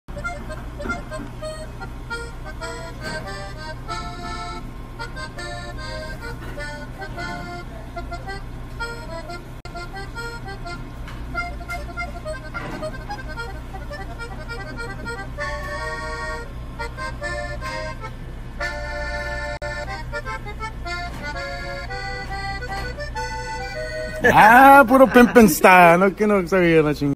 ACCORDION ABUSE ON PHONE
DA KRAZY PIMPZ
Eddie Jimenez Playing our latest single on his phone while eating before our show 🎶🎼🎶